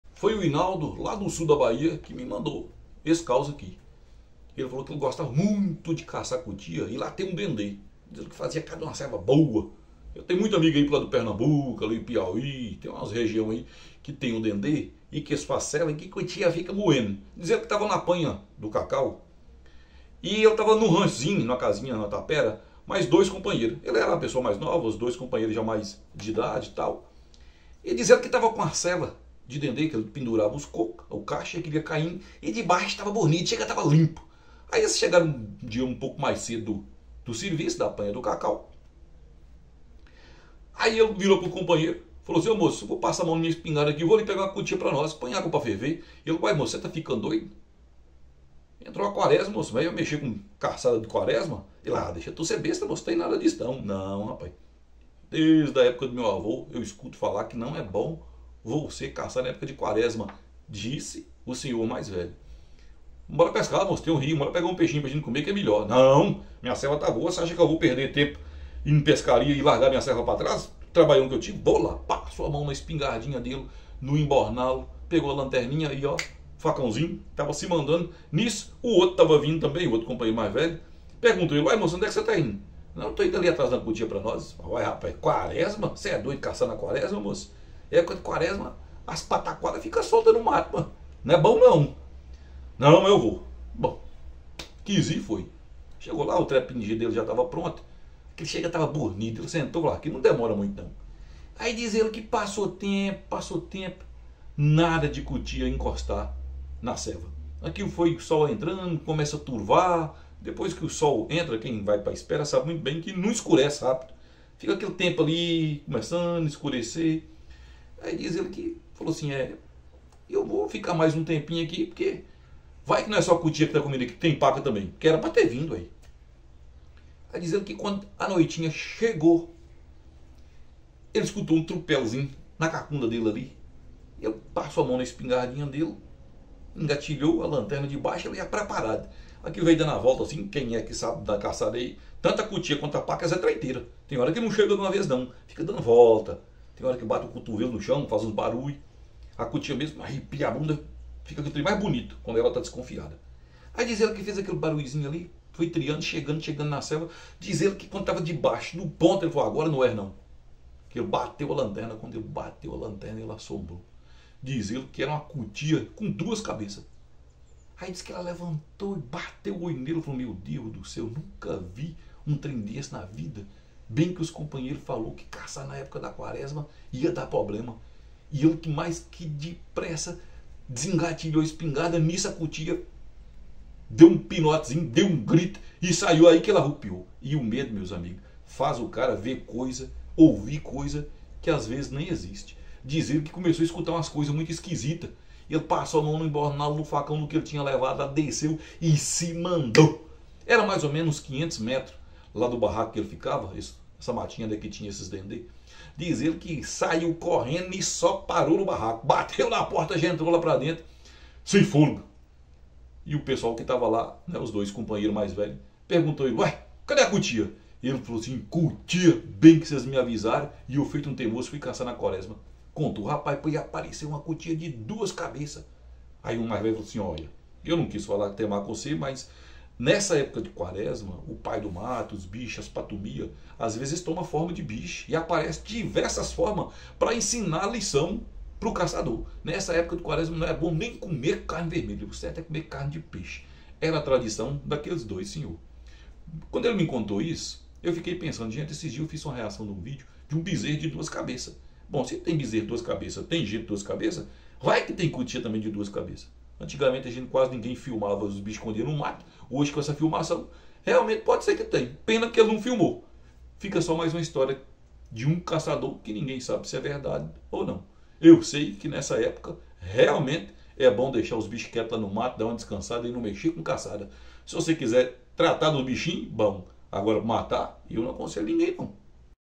Cutia de duas cabeças na ceva de dendê
Caçar na época da quaresma é sempre perigoso segundo as tradiçoes de alguns caçadores, existem muitos relatos de caçadores que tiveram que sair correndo do mato assombrado com coisas estranhas.